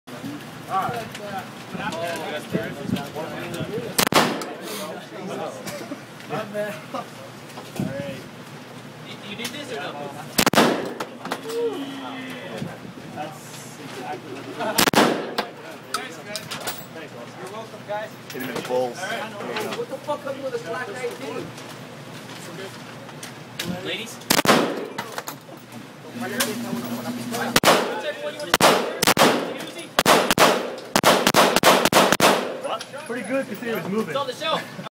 Shotgun shootin' in Vegas
Pumpin' dog the bounty hunter full of lead a few mins ago :-)